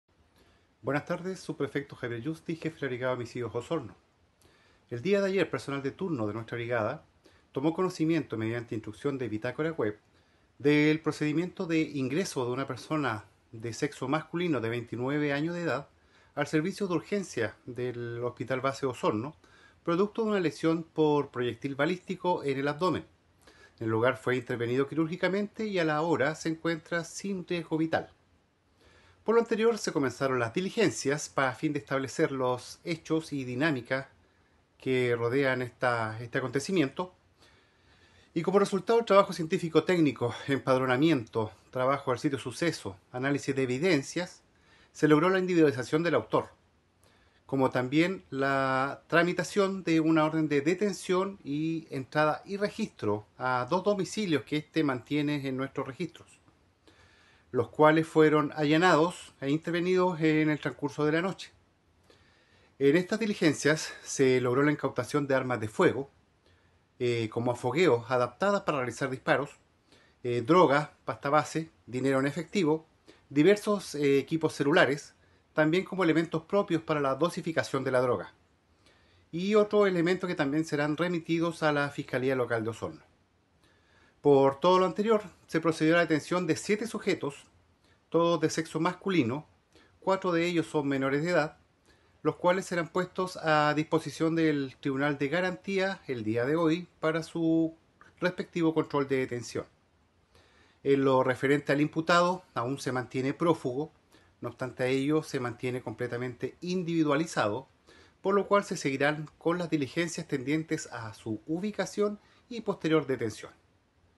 PDI incauta armas y drogas en Osorno: 7 detenidos
Un allanamiento de la PDI en Osorno dejó 7 detenidos tras el hallazgo de armas, drogas y otros elementos en dos inmuebles. Este operativo se originó tras un ataque a un hombre de 29 años, quien permanece en estado grave.